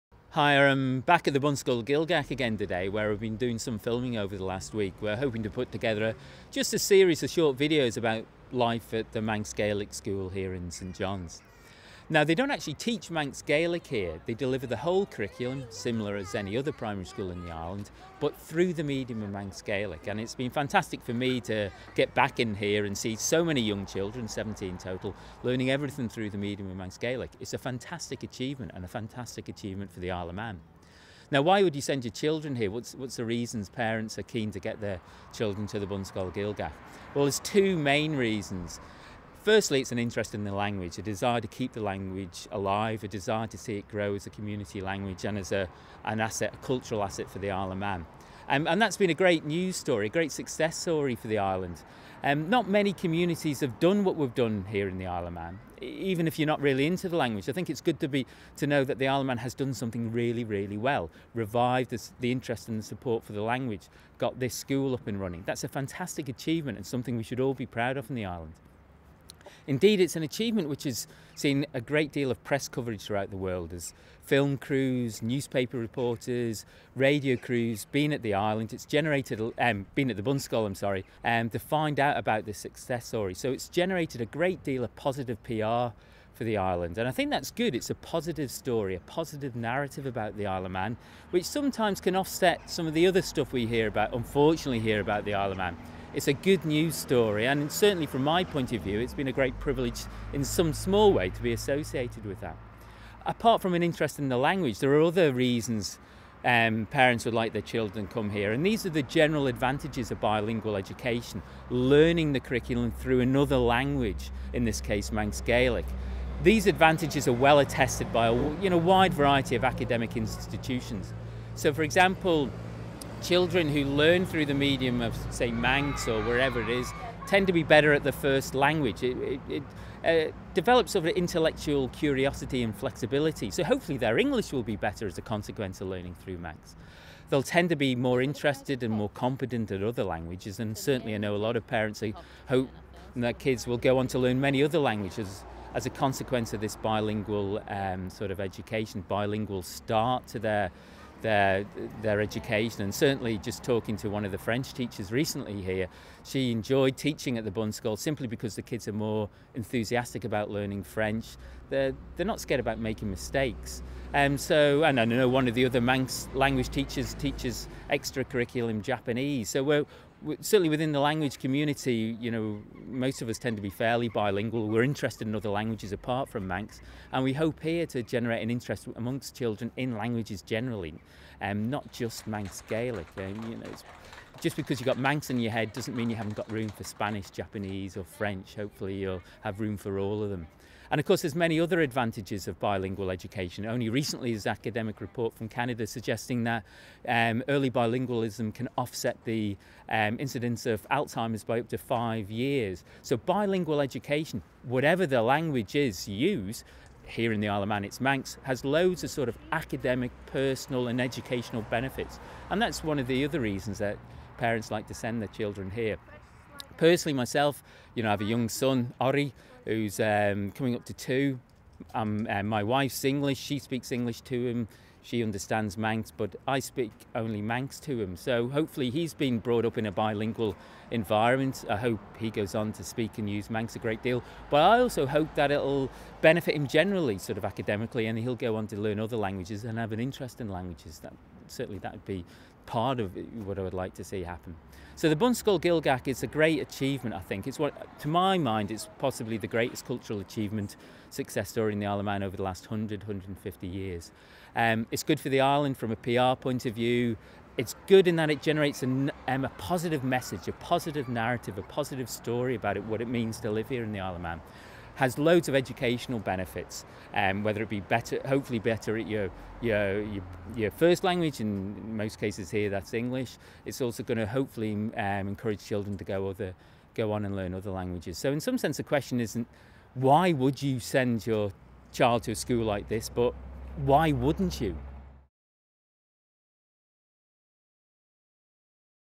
Adrian Cain at the Bunscoill
In this short video piece, which we put together after our day filming at the Bunscoill, I go over some of the key issues relating to bilingual education  as well as reiterating what the Bunscoill offers to the Island